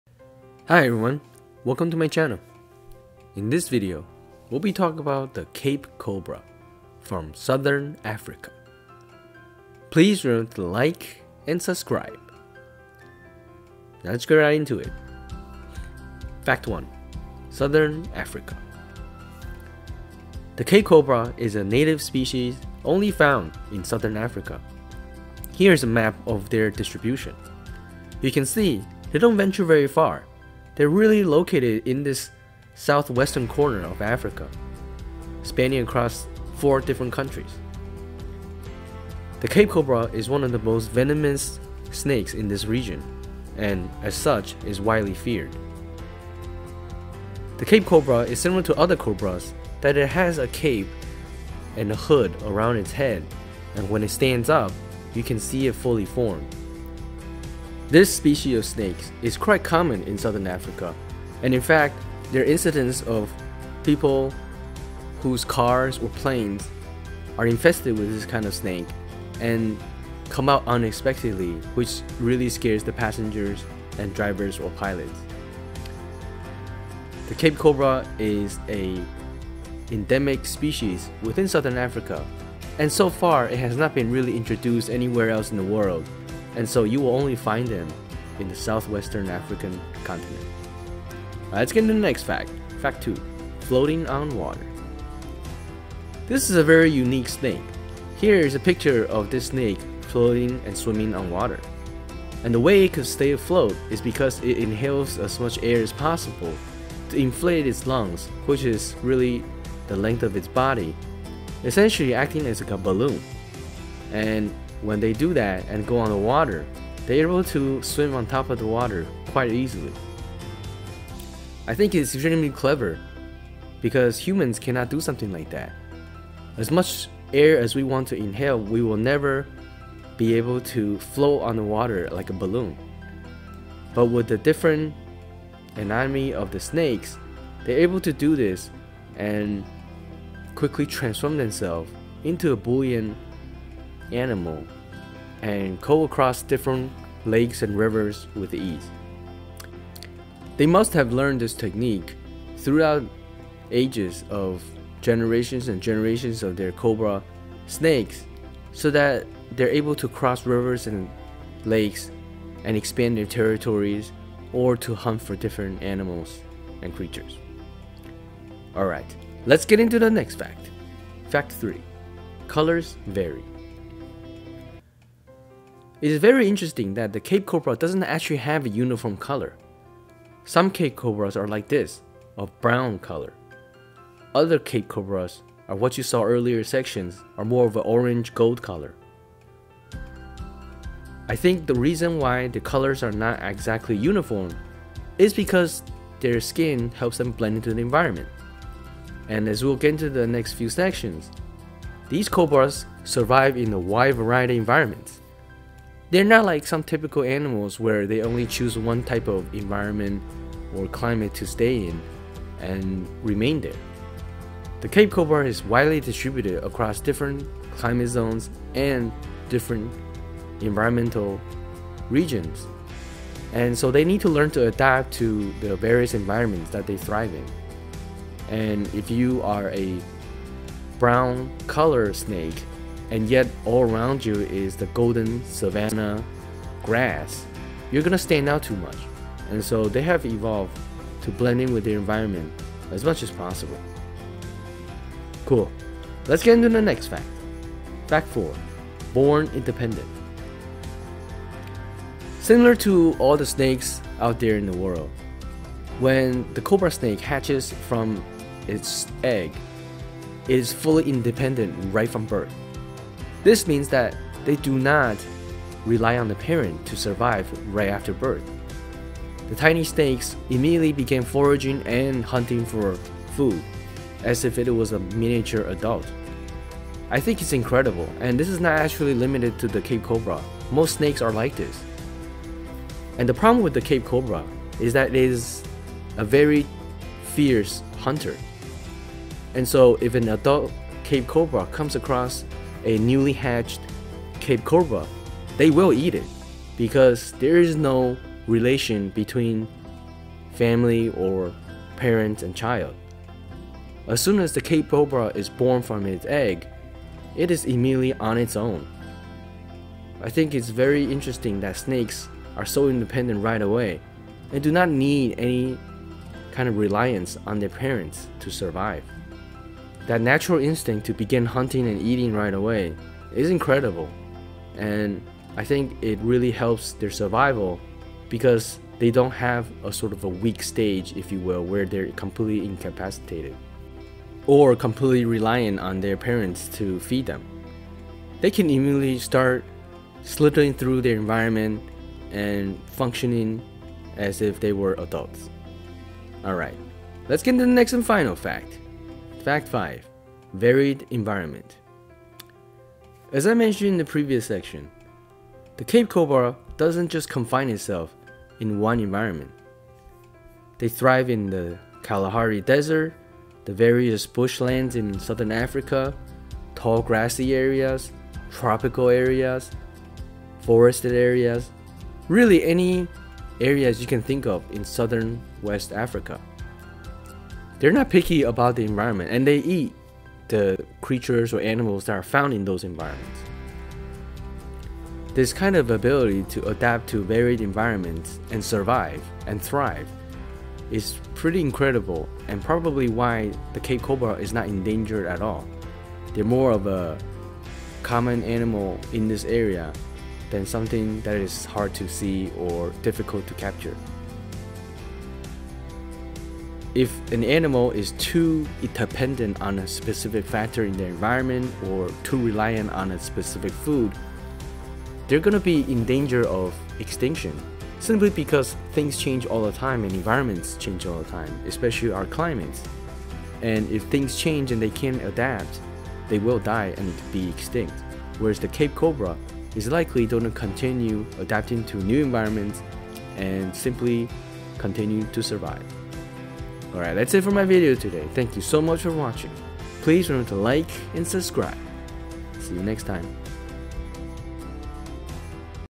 Surprising Things You May Not Know About Cape Cobras!
In this video, we'll be discussing the Cape Cobra, one of the most deadly snakes in Africa. We'll discuss its biology, distribution, and toxicity.